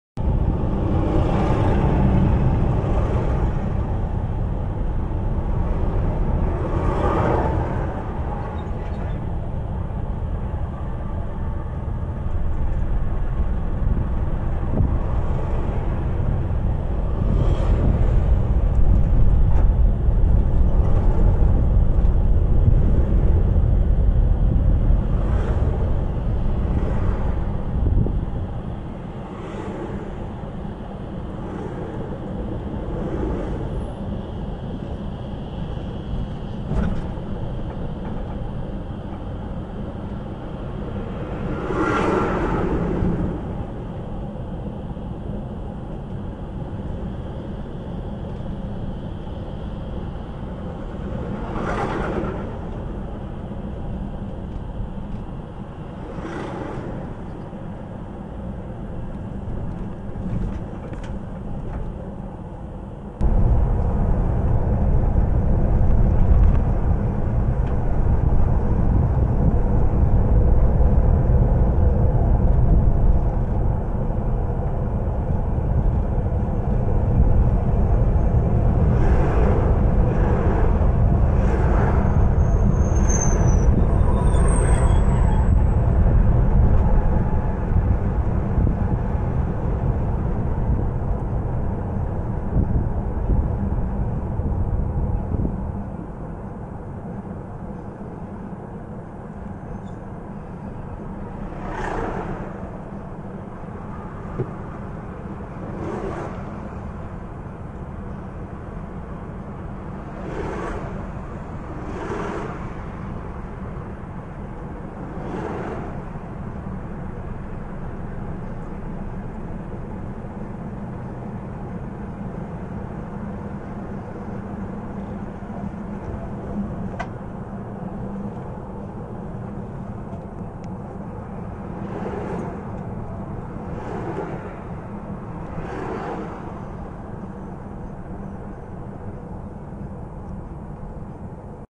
Road to Chefchaouen (Morocco)
Road to Chefchaouen (Morocco), ovvero la strada per la città blu del Marocco, Chefchaouen, tra natura, laghi e bei paesaggi. la perla blu